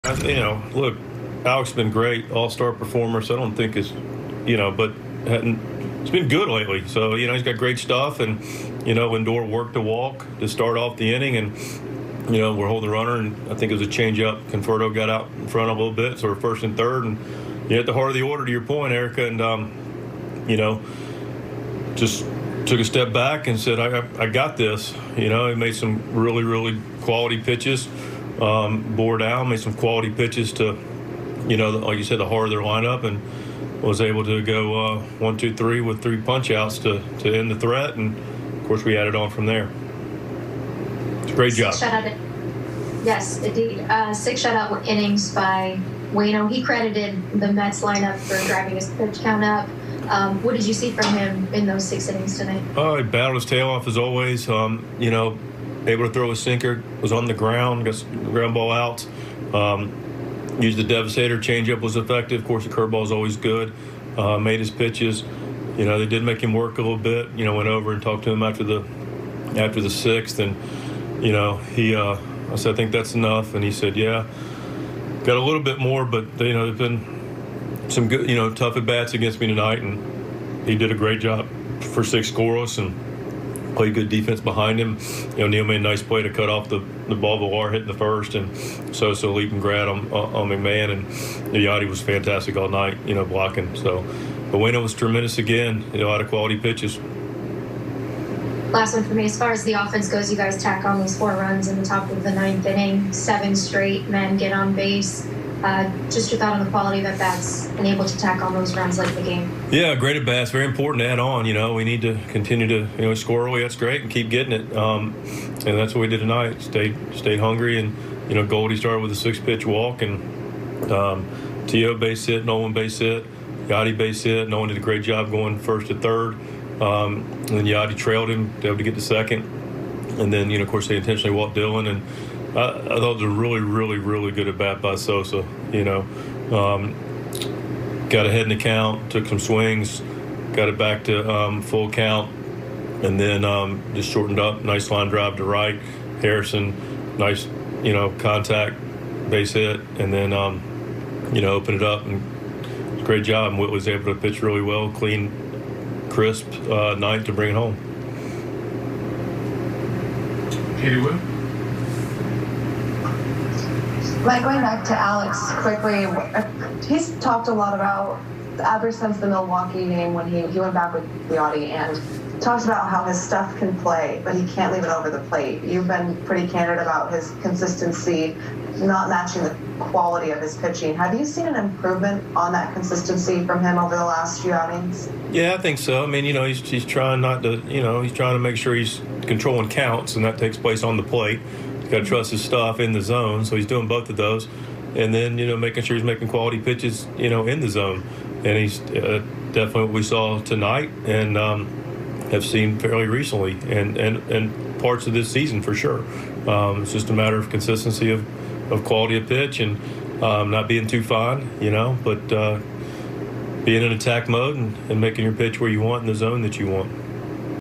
Mike Shildt on Alex Reyes: 'He's got great stuff'
Mike Shildt on Alex Reyes' work in the eighth: "He just took a step back and said 'I got this.' He bore down and made some quality pitches to the heart of their lineup."